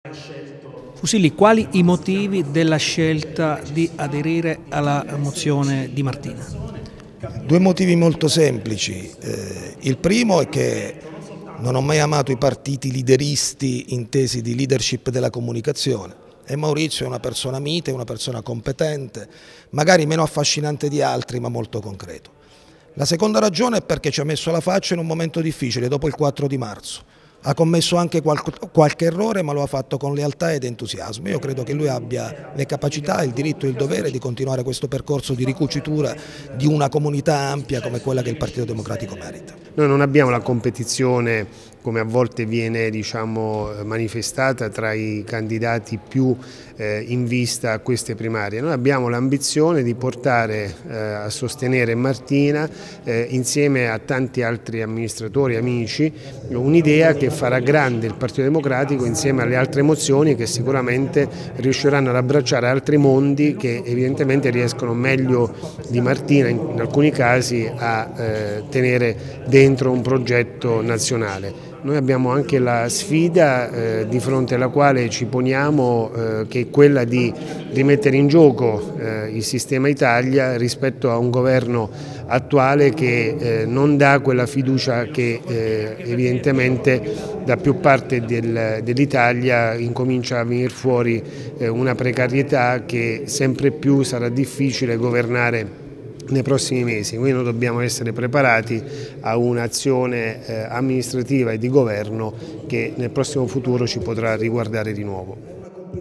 Pd: presentati i candidati di "Fianco a fianco Pescara per Martina"
Il coordinamento provinciale della Mozione di “Fianco a fianco – Pescara per Martina” ha presentato alla Stampa i cinque candidati all’Assemblea nazionale del Partito Democratico che sostengono la candidatura di Maurizio Martina alla guida del Partito Democratico. Si tratta di Gianluca Fusilli, Paola Marchegiani, Antonio Di Marco, Romina Di Costanzo e Daniele Giangiulio